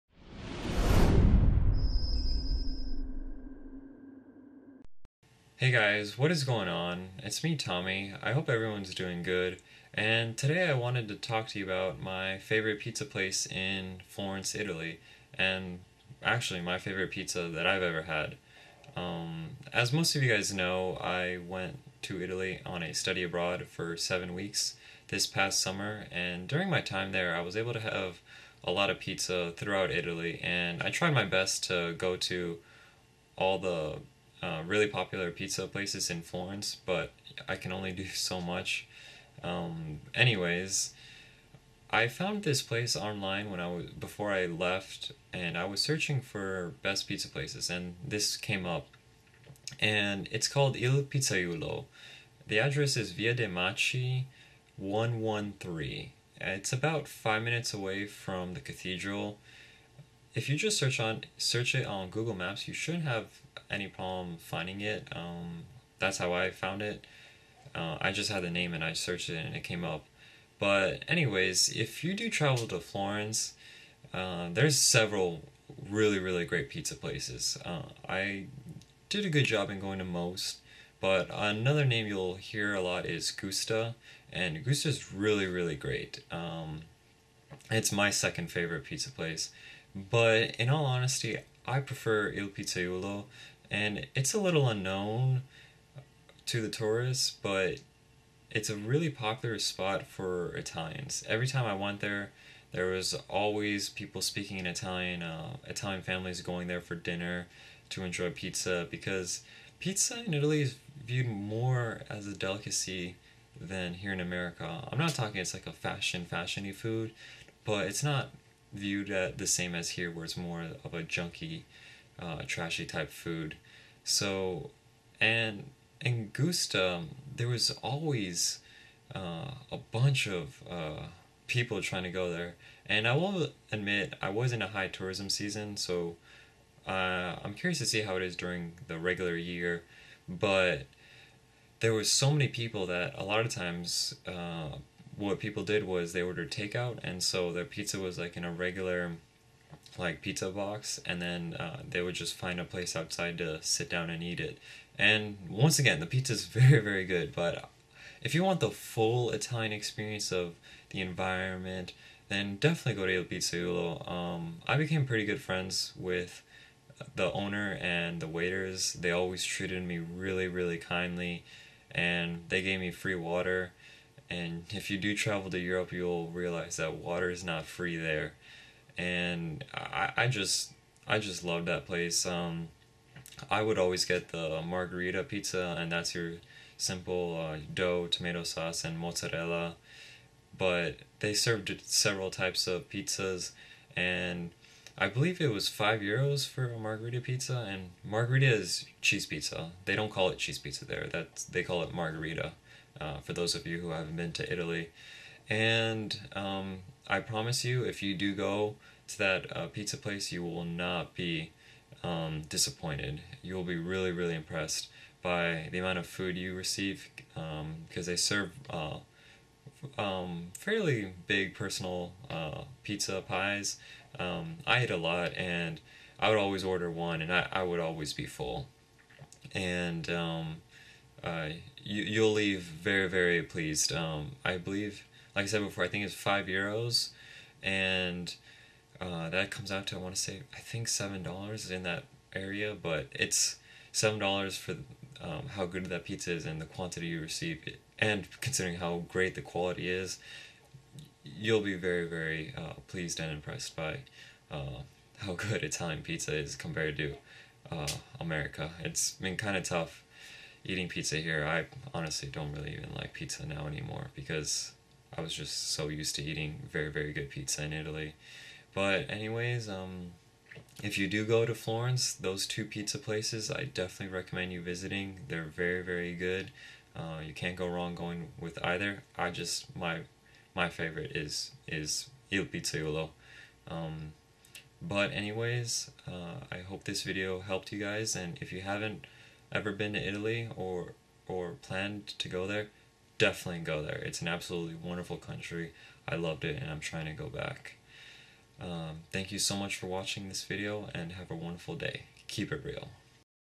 Best Pizzeria in Florence, Italy
Hey guys! Like I said in the video I couldn't eat at every single pizzeria, but this is my personal favorite pizza place. The name is "il Pizzaiuolo" and the address is Via de Macci, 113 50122. Hope this video helps and have a great day! :)

If you want to see the video where I talk about my experiences living in Florence for seven weeks that watch the video below! Thanks again for all the support.